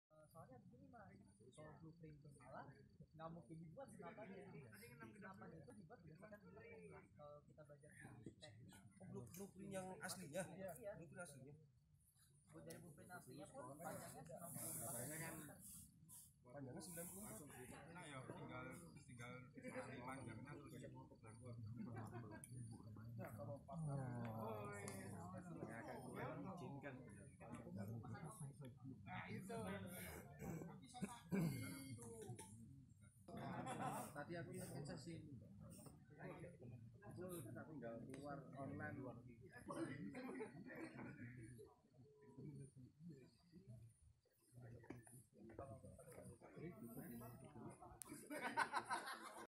Jambore WW2 Reenactors di Waktu Malam
Acara Jambore WW2 Reenactors ke-2, yang digelar pada tanggal 20-21 Juli 2019 di Bukit Moko (Puncak Bintang), Cimenyan, Bandung.